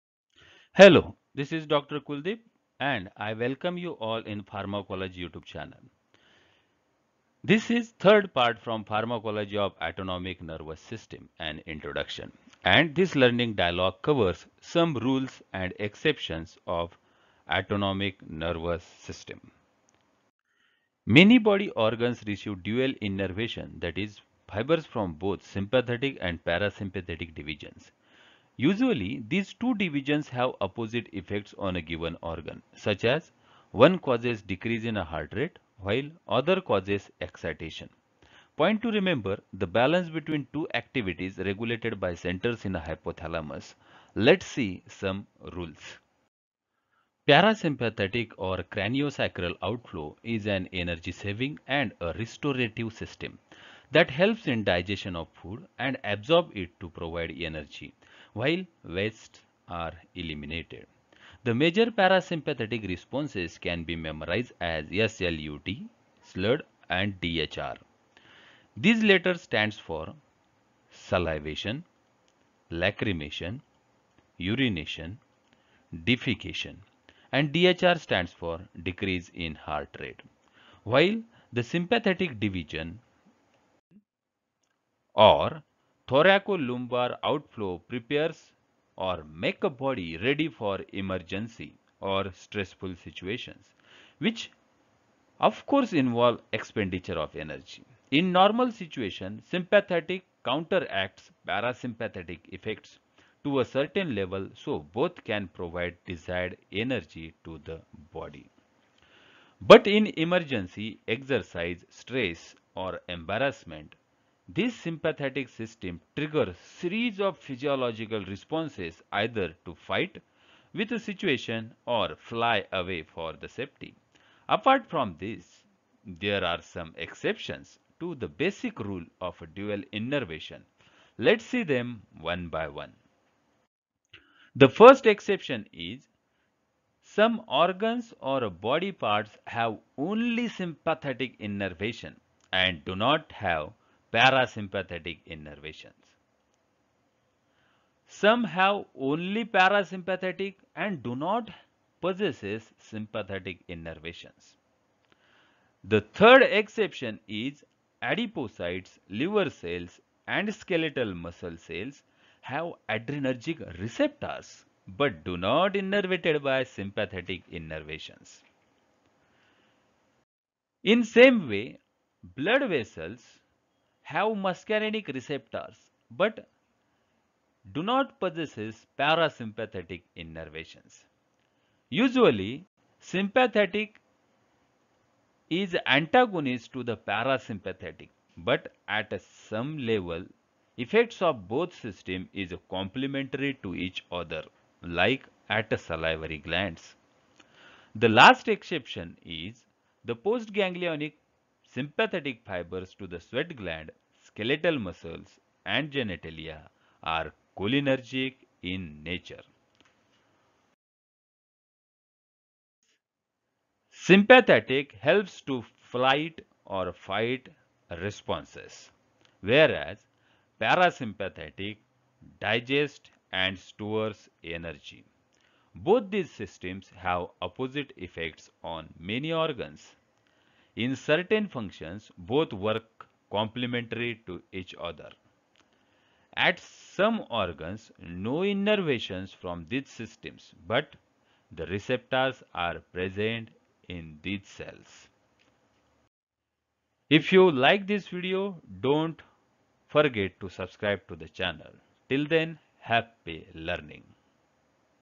Pharmacology of ANS Introduction Part 3 Rules and Exceptions
Pharmacology of ANS Introduction Part 3 Rules and Exceptions by Dr. Kuldeep Bansod
Many body organs receive dual innervations from fibers from sympathetic and parasympathetic divisions. Usually, these two divisions have opposite effects on a given organ such parasympathetic decreases heart rate while sympathetic increases it.
Here some rules and exceptions are described in a simple and interesting way.